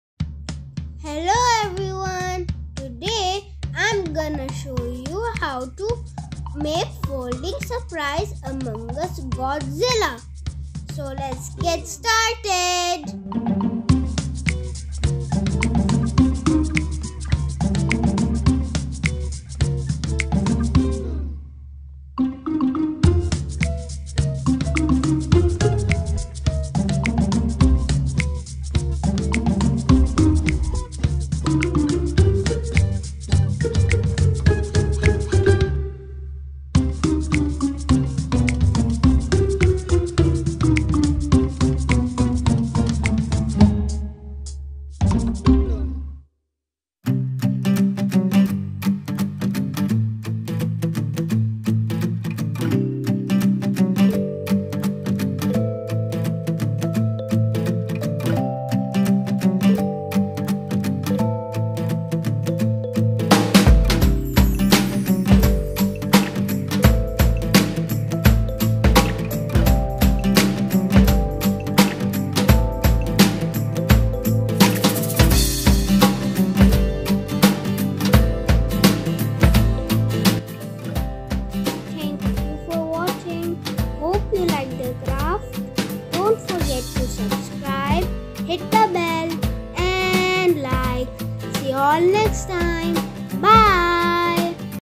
Folding Surprise Among Us Godzilla! ROARR!!!!!
Hello! Today Affan is making Folding Surprise Among Us Godzilla!!
These are the things which Affan used.
Markers: Blue, Dark Blue and Black ,Grey
Color Pencil: Light Blue